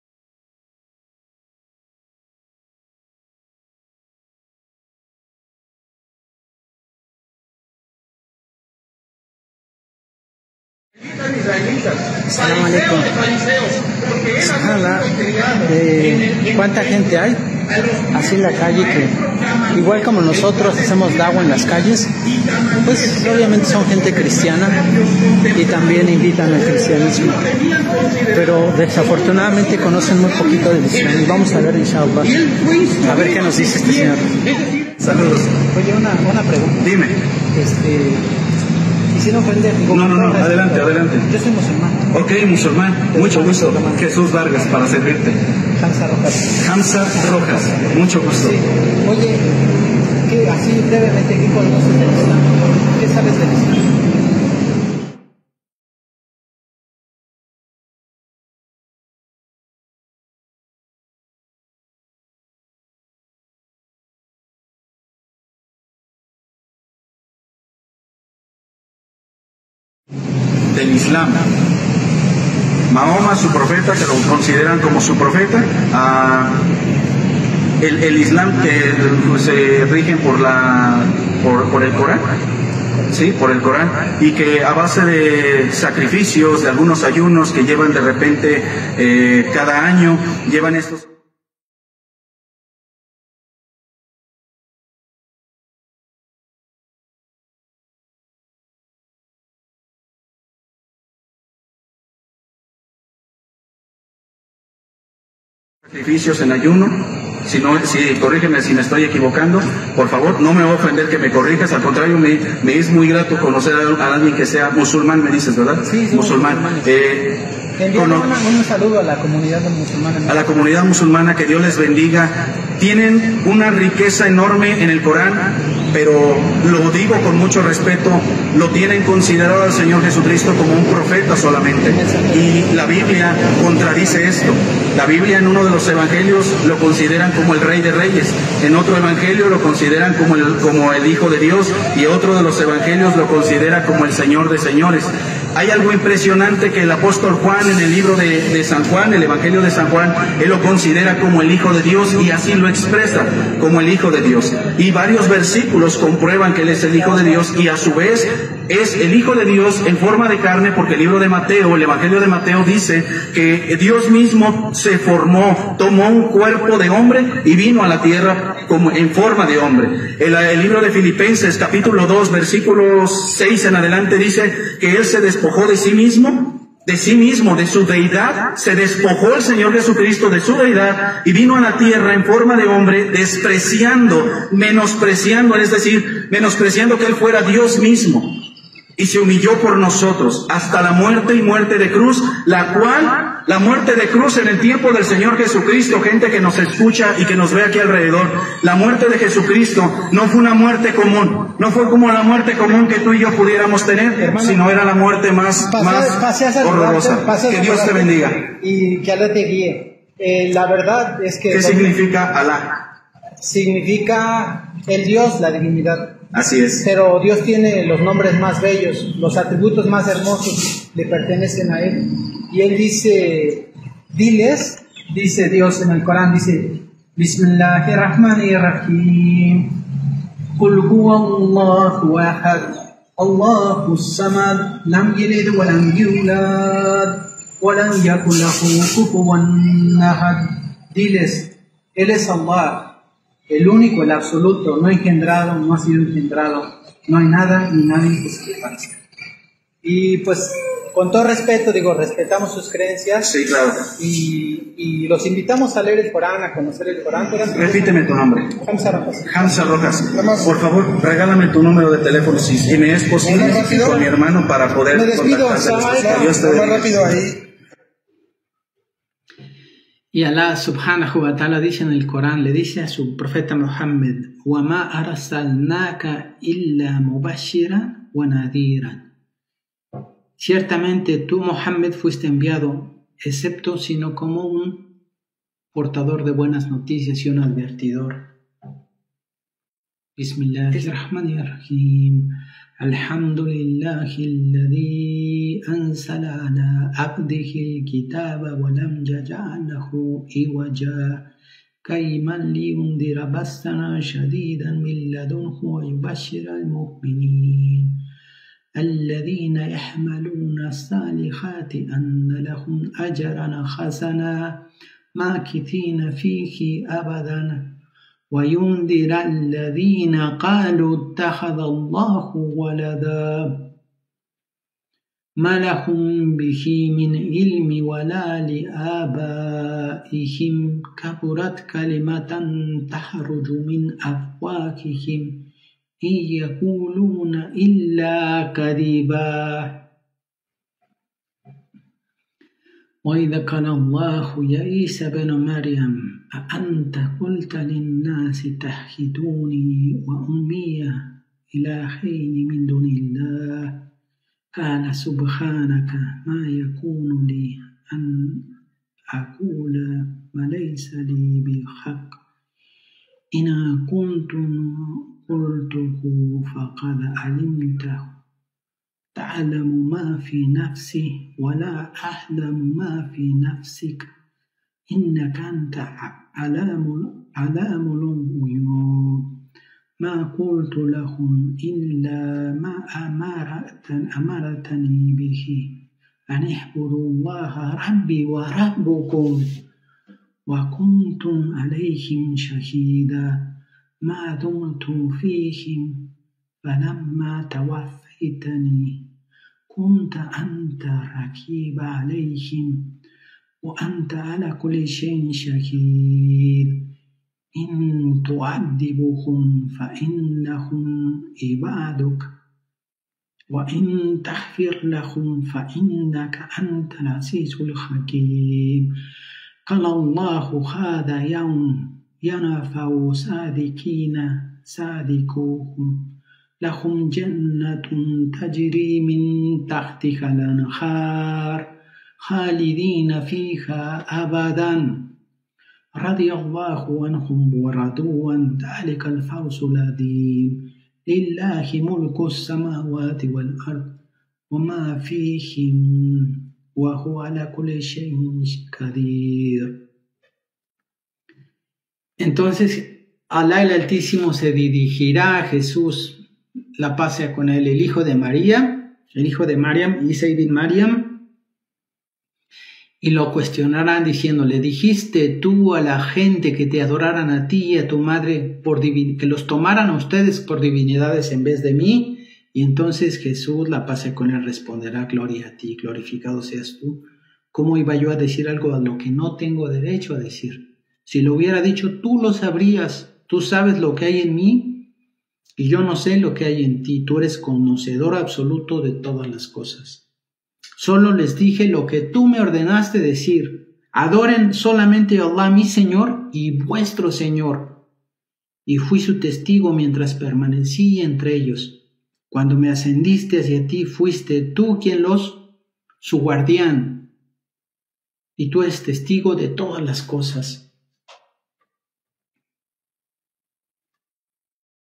PLÁTICA ENTRE UN PASTOR CRISTIANO Y UN MUSULMÁN
حديث بين قس نصراني
وبين الداعية / حمزة روخس
في كيريتارو بالمكسيك